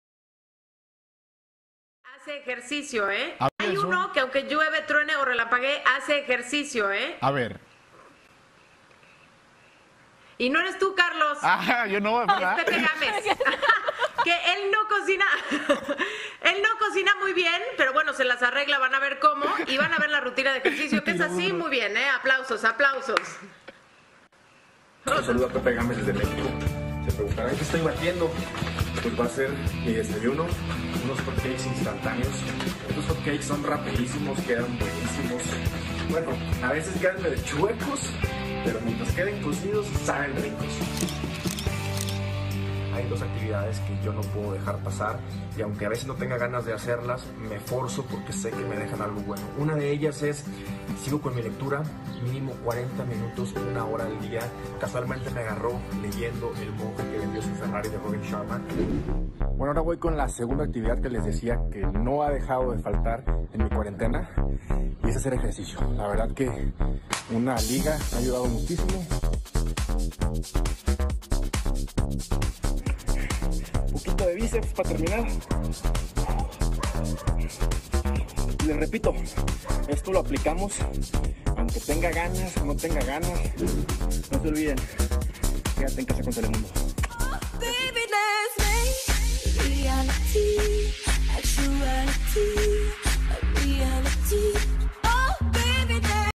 Pepe Gamez enseña su receta Favorita y su rutina en Quarentena
el gran actor @ppgamez le cuenta a @jimenagallegotv su 2 cosas importantes para vivir esta
y nos enseña su receta preferida.